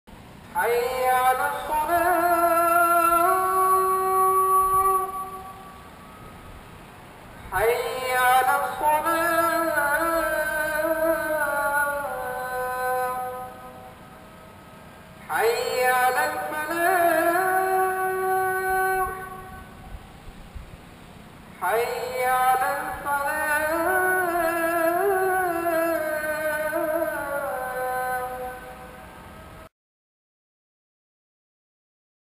Azaiba Azan 2014
evening call to prayer from my rooftop